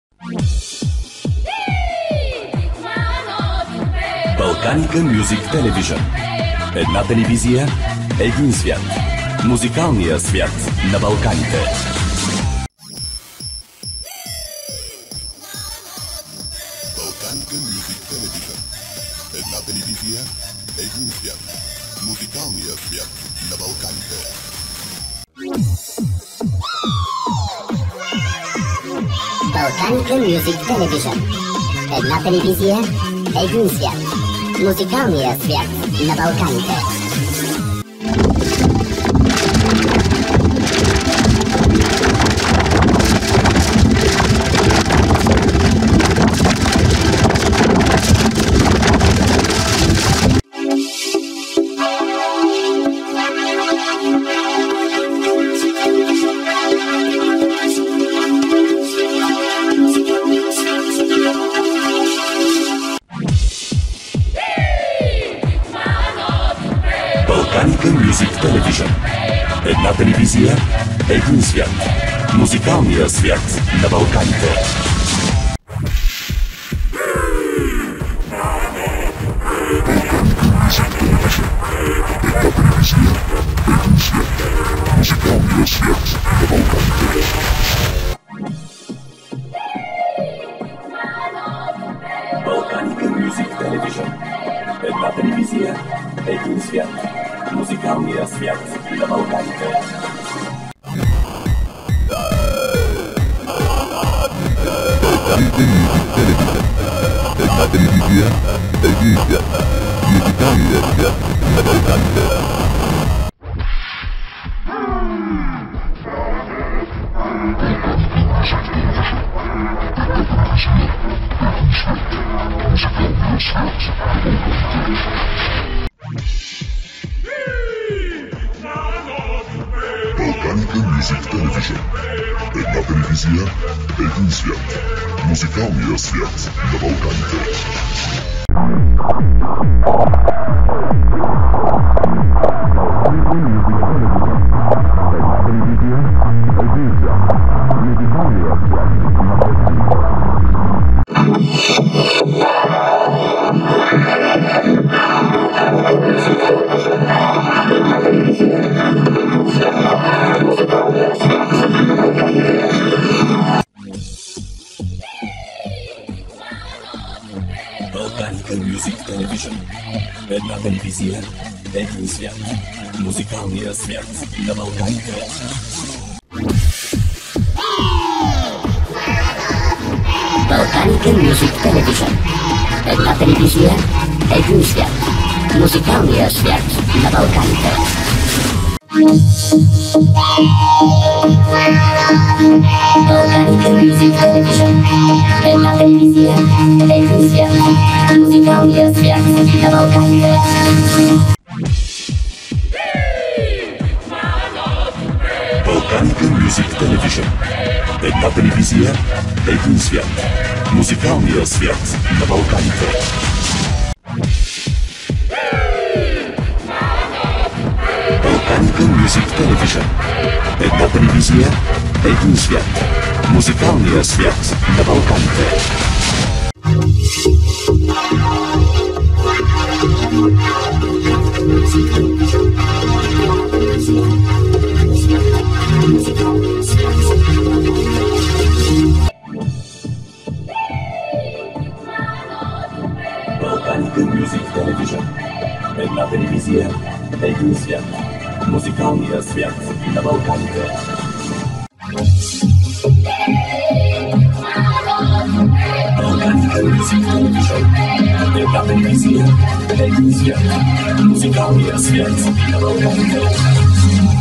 Balkanika TV BG Intro (2005 - 2013) | Effects | EXTENDED V2
Effects:
0. Normal
1. Timecode Major
2. G-Major 1
3. 1.0 Clouds Day DUH
4. YouTube Flanged Saw
5. G-Major 16
6. X-Terion Effect 1.0
7. Full Chord 4.0
8. Conga Busher 1.1
9. X Caught A C0r0n@v!ru$
10. I Sused X
EXTENDED:
11. Electric Water
12. Adorabat Mega Ensemble
13. Synthetic Speech
14. Green Yellow Red Show
15. YellowHeliumChorded
EXTENDED V2:
16. G-Major 100
17. Extra Scary G-Major
18. Blue Sea Flanged Pulse
19. BlackFridaymaj Vocoder
20. RoyalPurpleFlangedSawChorded